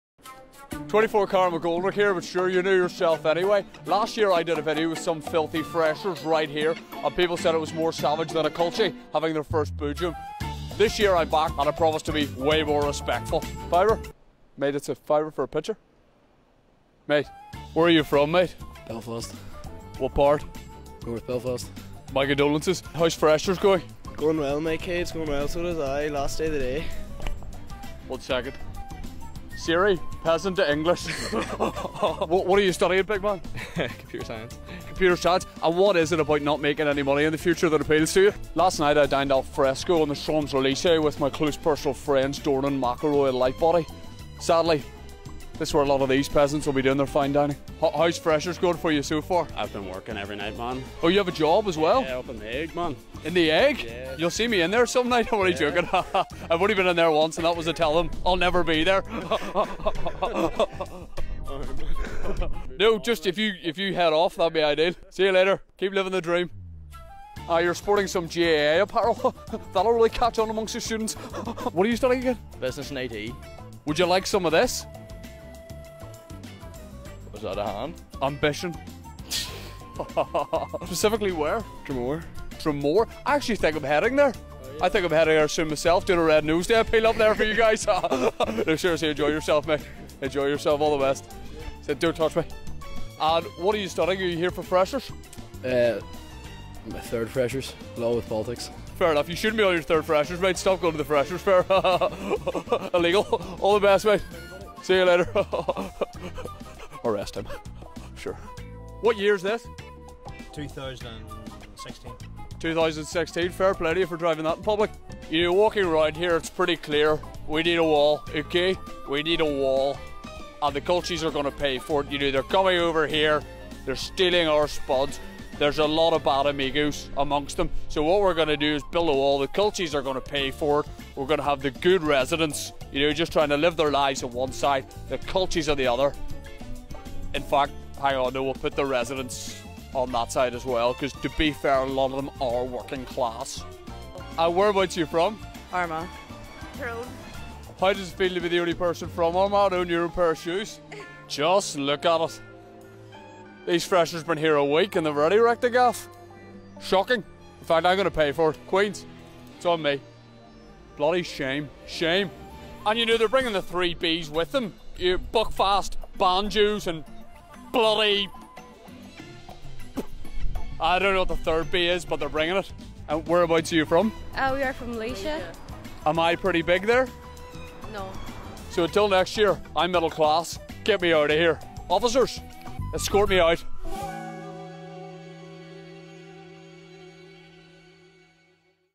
McGoldrick's ReFresher
24 Carat McGoldrick hits the Holylands and attempts to get into the mind of students having their first week of uni life...